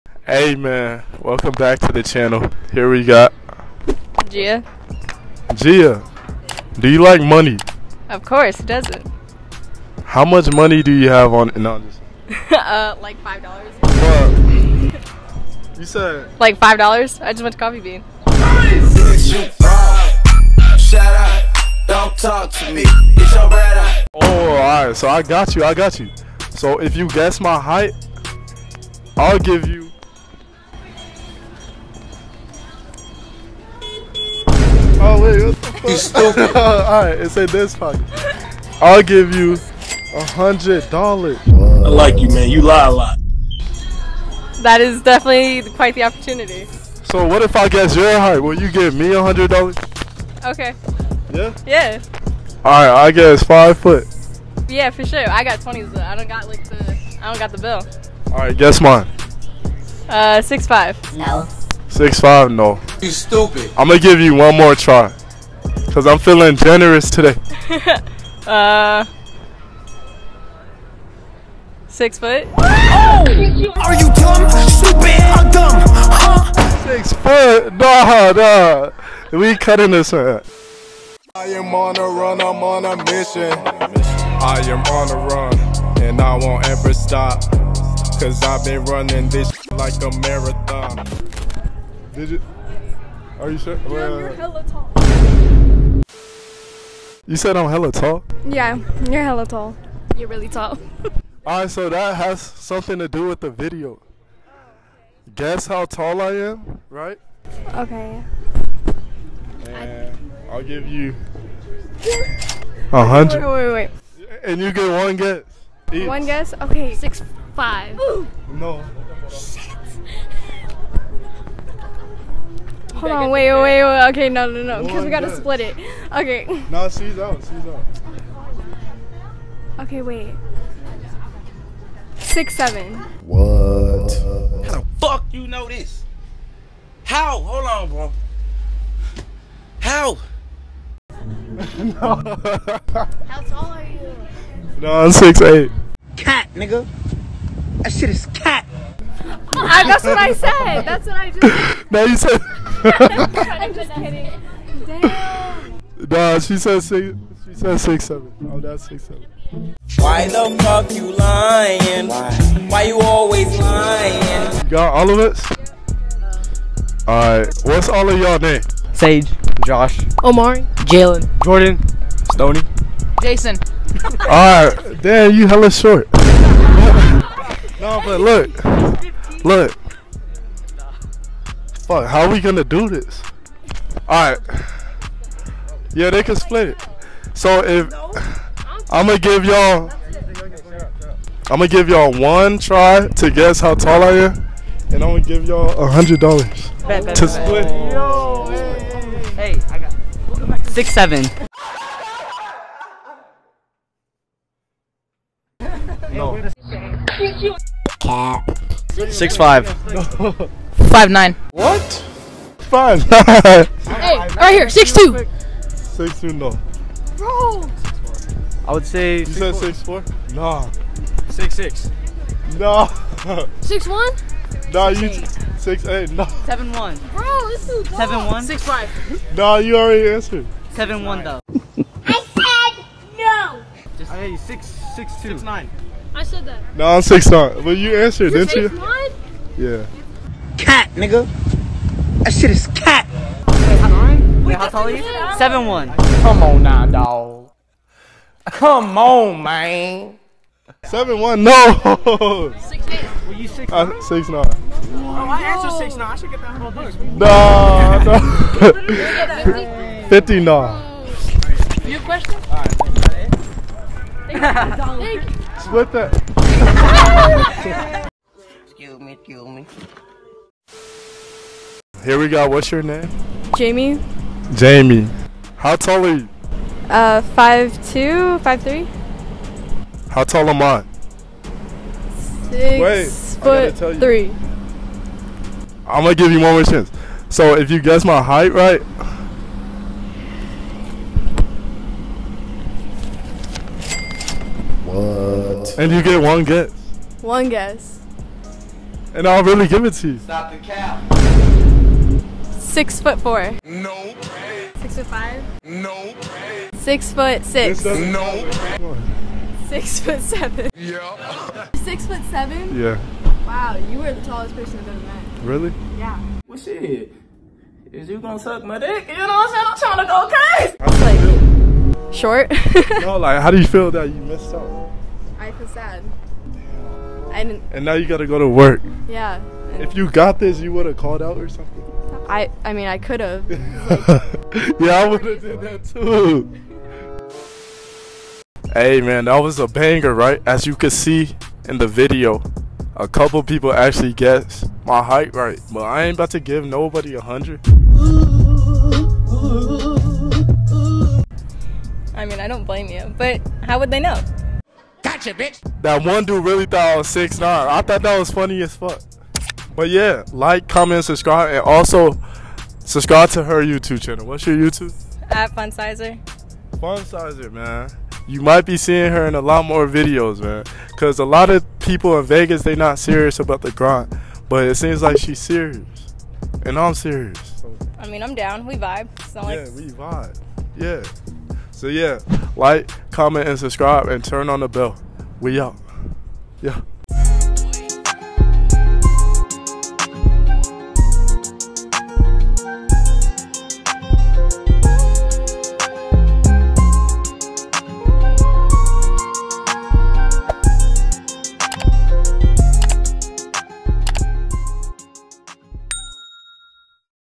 GUESS MY HEIGHT WIN $100 | PUBLIC INTERVIEW
I had people guess my height to win a $100.